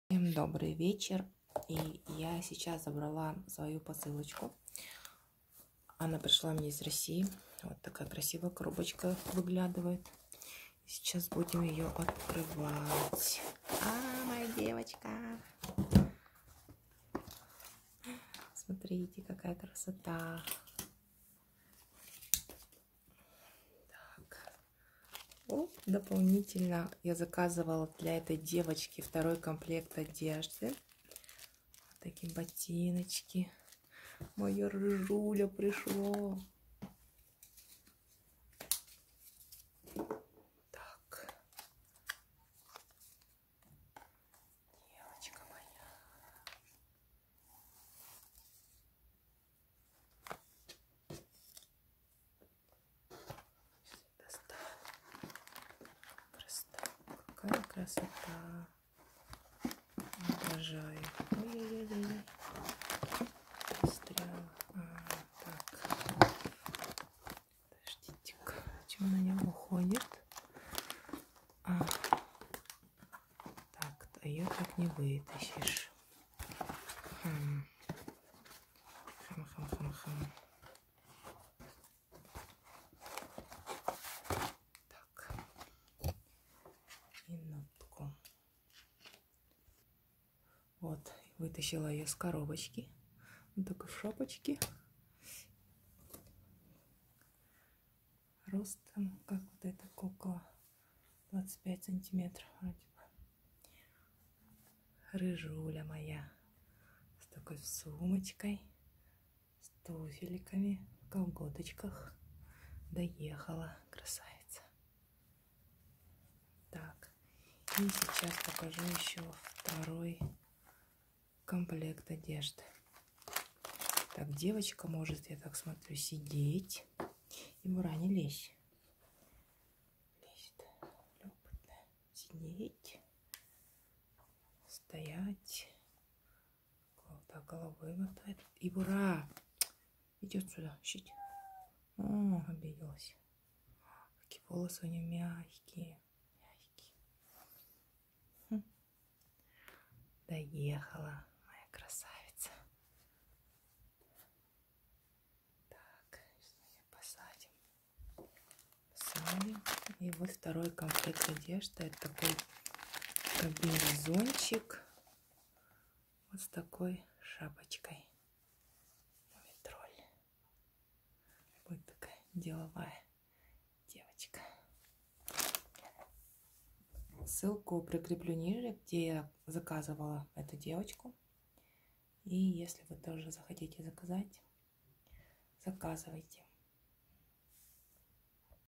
Распаковка посылки из России. Кукла интерьерная от
Стоимость куклы 3000 руб.
Второй комплект одежды 700 руб.
Доставка до Кореи 900 руб.

Волосы у девочки козочка ангорская. На ощупь мягкие. Стоит самостоятельно, но падает. Поэтому лучше ставить на опору. Может сидеть. Ручки гнутся.